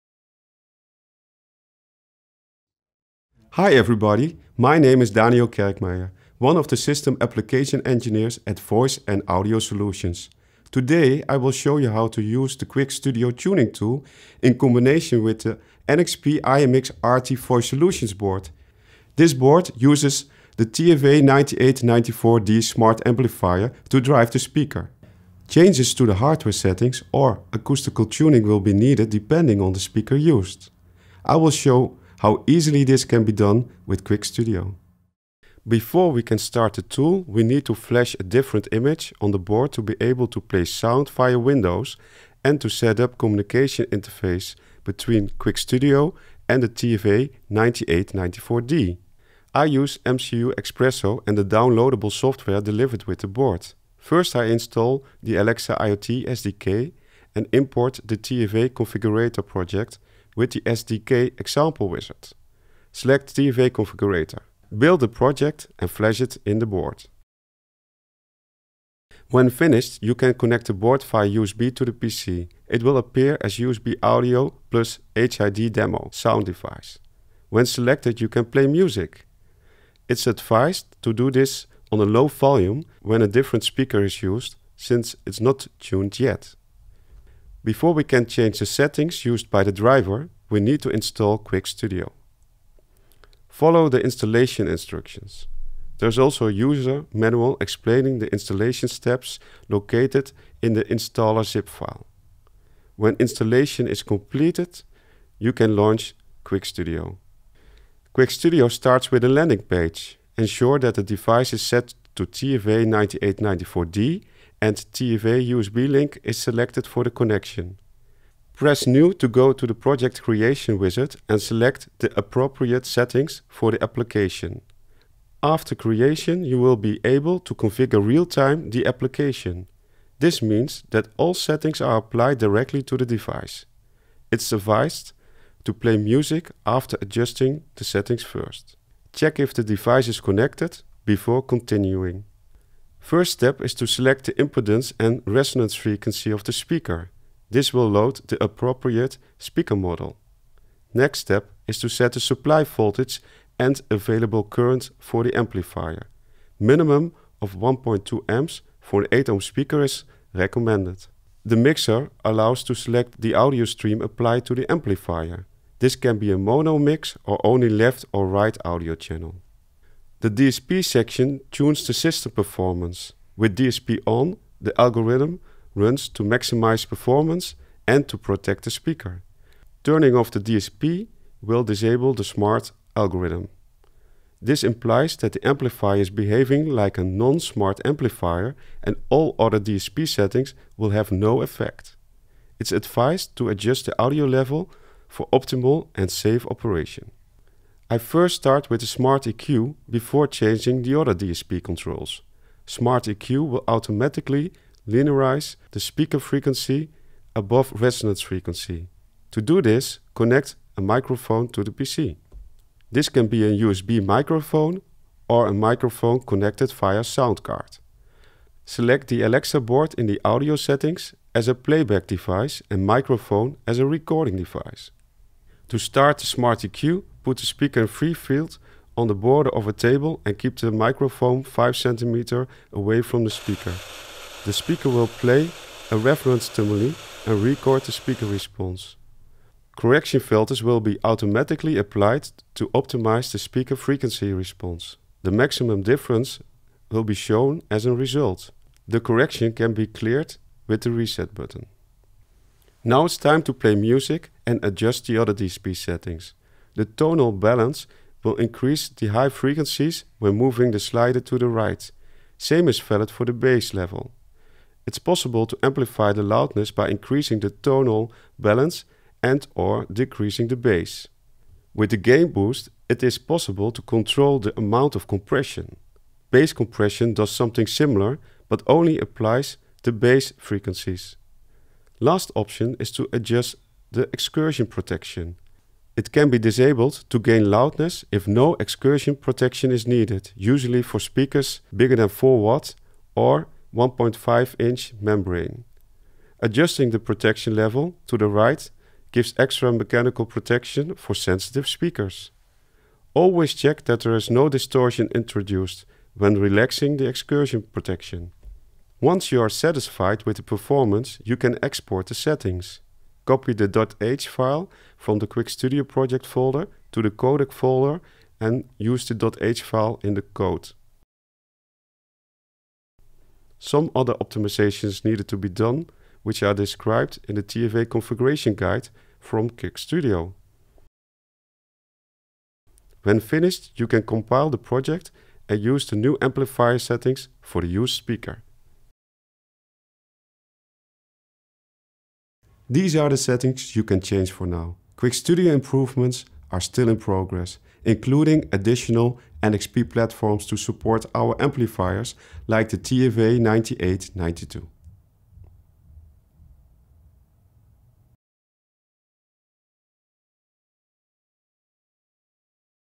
How To Use QuickStudio with IMX Alexa Voice Solution to make changes to the TFA9894 amplifier
QuickStudio is needed to change the amplifier settings when a different speaker is used, when hardware changes apply like power budget / external supply or when the audio characteristics need to be changed.

This video guides you through the steps needed and explains the functionality of QuickStudio.